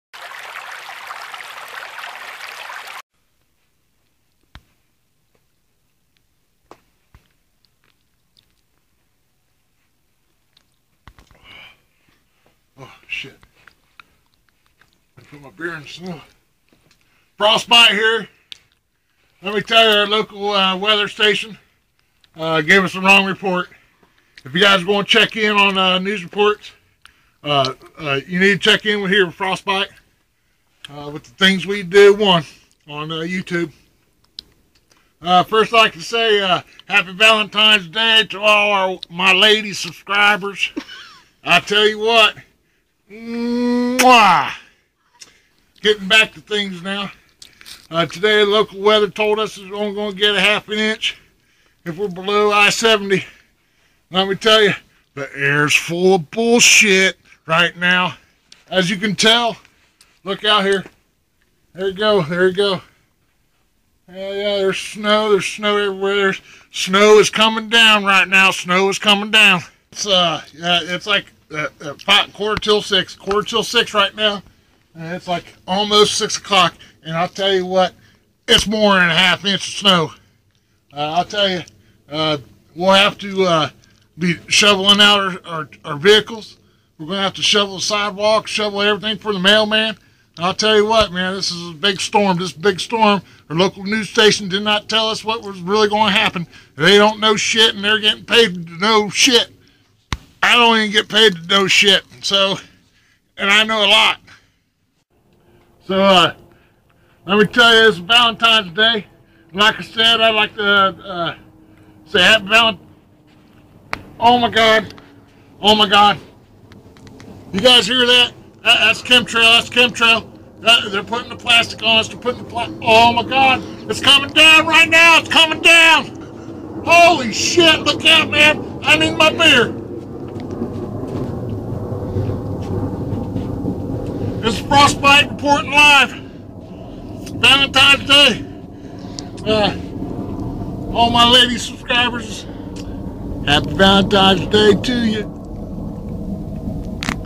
Happy Valentines Day from "Frostbite" and the thangswedo1 channel
The weather man predicted the weather wrong here in Ohio...AGAIN!! 1/2 inch my arse!! Here is a Happy Valentines day wish from "Frostbite" to all the lady subscribers here on youtube.